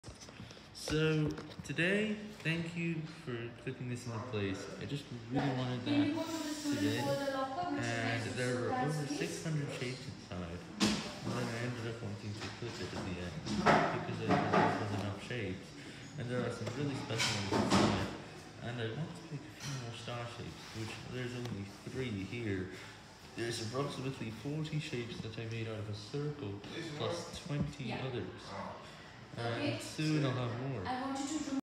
The shapes bag has been closed.
I wanted to close my shape bag after there were hundreds of shapes inside it.